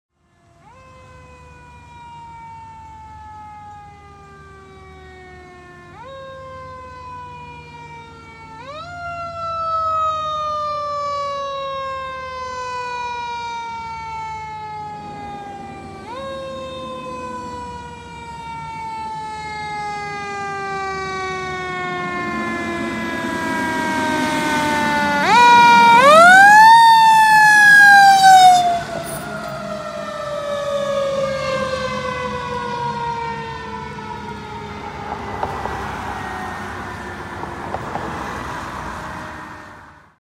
Winona Fire - Engine 2 Responding - 8-16-18
WFD Engine 2 responding to a medical emergency. Thanks for making sure the Q worked, Cap. Enjoy!